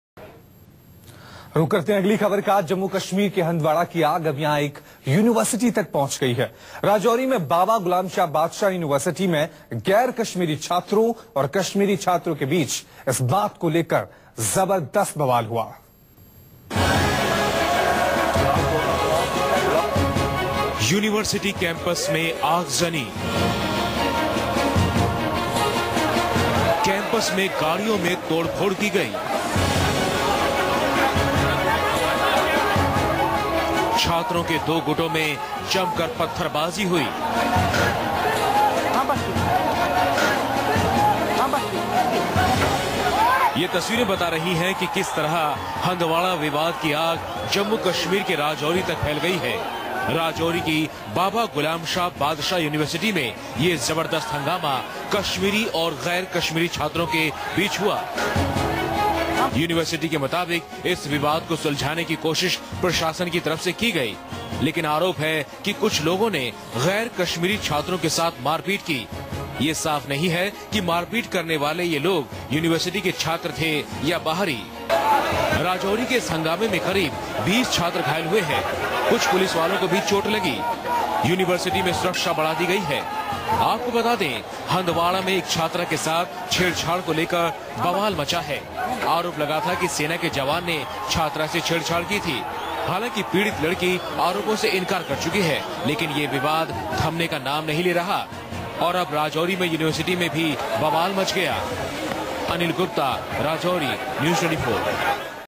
12 injured as students clash in BGSBU campus in Rajouri
12 injured as students clash in BGSBU campus in Rajouri